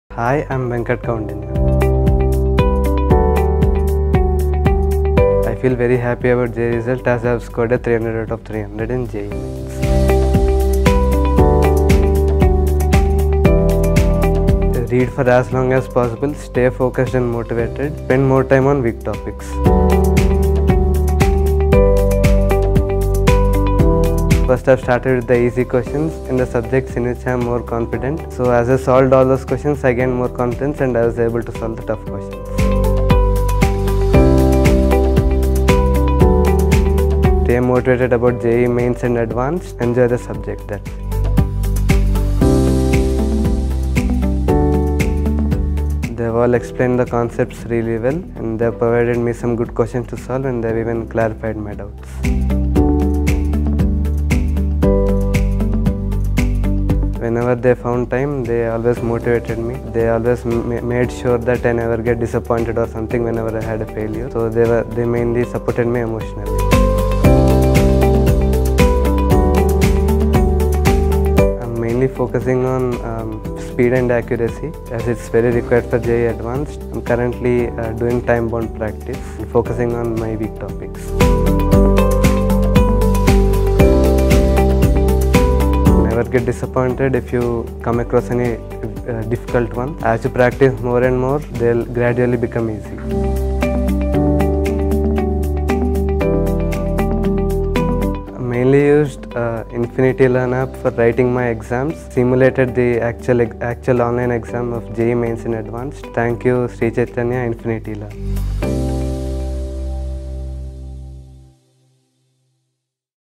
All India Rank 1 Venkat Koundinya || JEE Main Results 2023 || Sri Chaitanya
📌JEE Main 29th Jan Shift 2 Paper Solution Analysis 🎯NEET 2024 Paper Solutions with NEET Answer Key

📲IIT JEE Main College & Branch Predictor 2024 Welcome to this inspiring video about the JEE Main toppers from Sri Chaitanya. In this video, we will be showcasing the incredible achievements of students who have excelled in one of the toughest entrance exams in the country.

Our expert speakers will be discussing the success stories of JEE Main toppers from Sri Chaitanya, sharing their strategies and tips for success. We'll also be hearing from the toppers themselves, as they talk about their preparation journey, the challenges they faced, and how they overcame them.

If you're a JEE Main aspirant looking for inspiration and guidance, this video is a must-watch. Learn from the best and discover the secrets to cracking this challenging exam.

So, get ready to be motivated and inspired by the stories of these outstanding achievers.

Topics covered session:
All India Rank 1 Venkat Koundinya  || JEE Main Results 2023
0:00-0:21 How do you feel about your JEE main Results
0:22-0:35 Strategy for tackling difficult Questions in JEE
0:36-0:42 Tips for staying Focused and Disciplined
0:43-0:55 Teacher's role in JEE Preparation
0:56-1:07  Family preparation
1:08-1:53 JEE Advanced  Preparation and Strategy